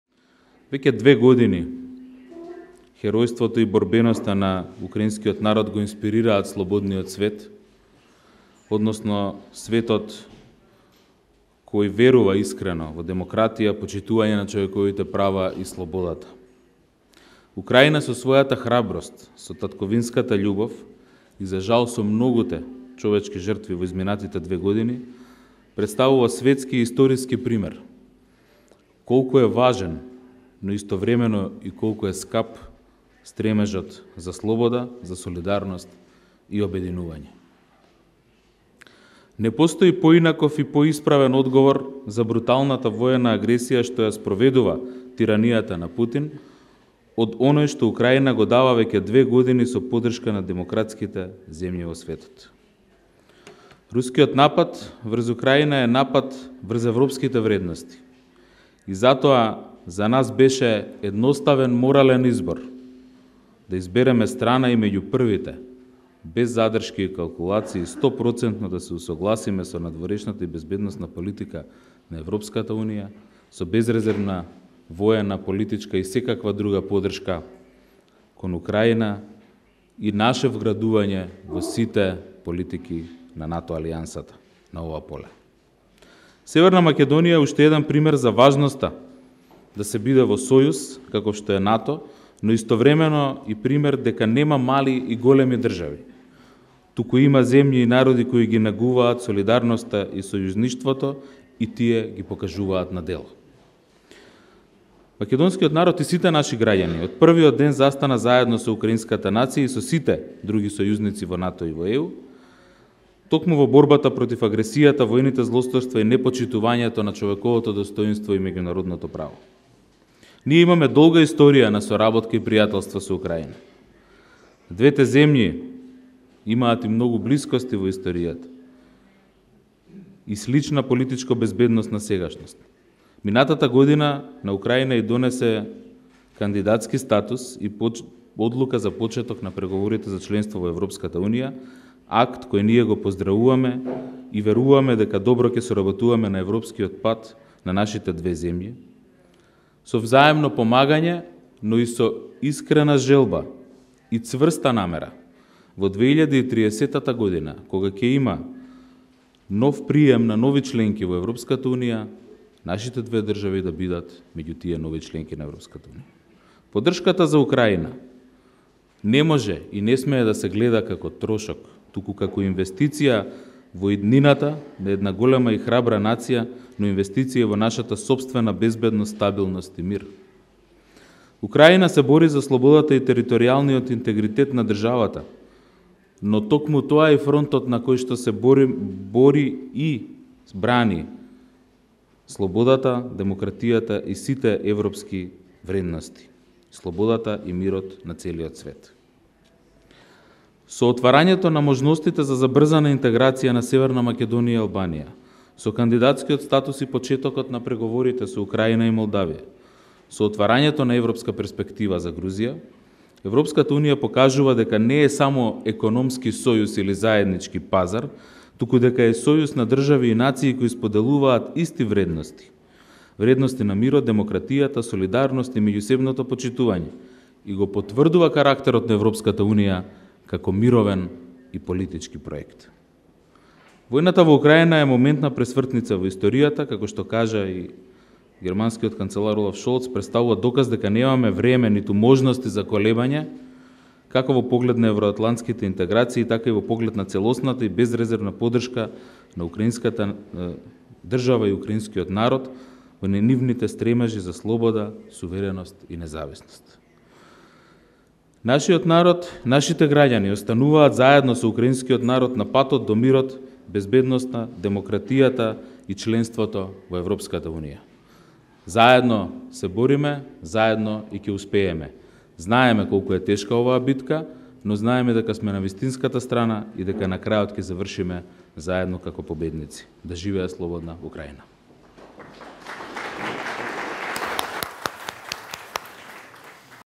Маричиќ: Рускиот напад врз Украина е напад и врз европските вредности
„Веќе две години, херојството и борбеноста на украинската нација го инспирираат слободниот свет односно светот во кој доминираат демократијата и почитувањето на човековите права и слободата. Украина, со својата храброст, со татковинската љубов и за жал со многуте човечки жртви во изминатите две години, претставува светски и историски пример колку е важен, но истовремено и колку е скап, стремежот за слобода, за солидарност и обединување“, изјави вицепремиерот за европски прашања, Бојан Маричиќ, на мултимедијалната конференција „Украина – две години хероизам“, во организација на Амбасадата на Украина во соработка со ЦИВИЛ.

монтажа: Ариан Мехмети

www-civilmedia.mk